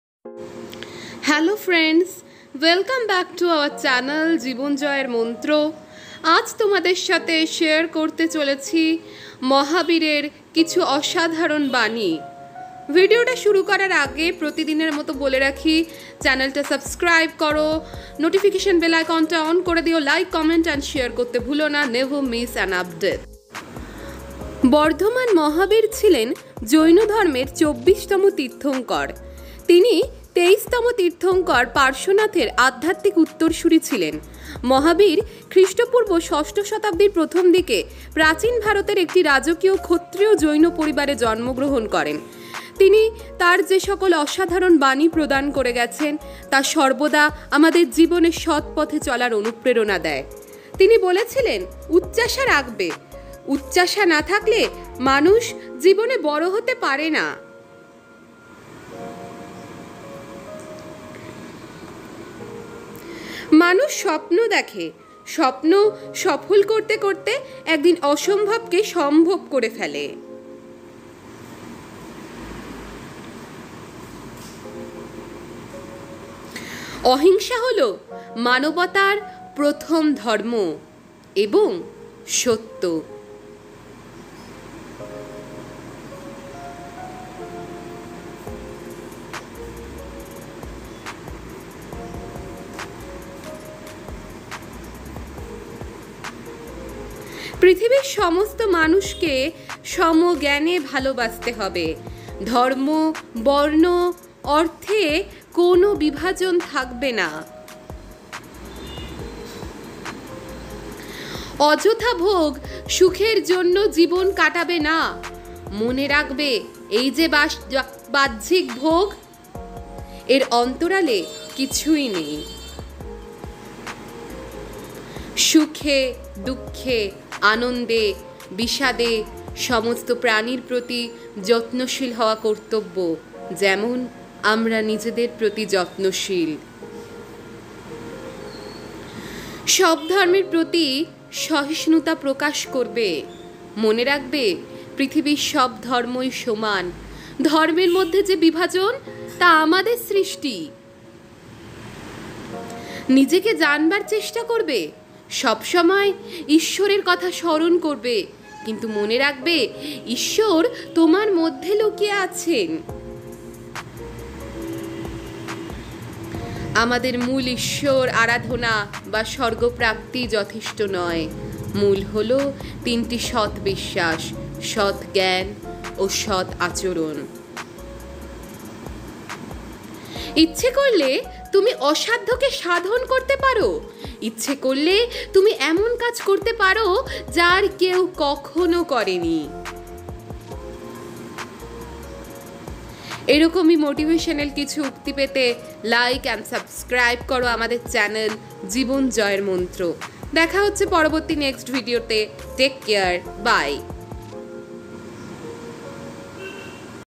| মহাবীরের কিছু অসাধারণ বাণী | Mahavir Quotes |MotivationalQuotes | @jibanjoyermantra8754 |
Today We will say some life changing Motivational Quotes of Mahavir. Follow our YouTube Channel. We will be very Glad if we can change your Life. Always be Positive and Motivated.